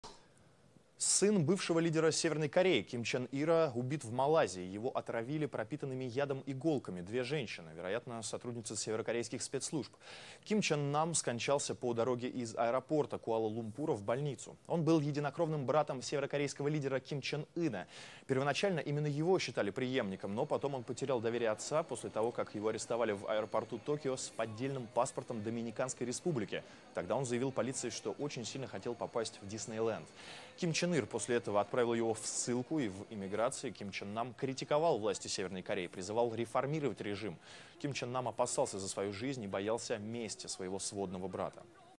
Малайзияда Түндүк Корея лидеринин агасын өлтүрүп кетишти
Ким Чен Ындын агасы 13-февралда Куала-Лумпур аэропортундагы кол салуудан каза болду. Ал узак мезгил чет өлкөдө жүрүп, Түндүк Кореядагы үй-бүлөлүк бийлик үстөмдүгүн сынга алып келгендиги менен белгилүү болгон. Анын 45 жашта болгондугу айтылууда.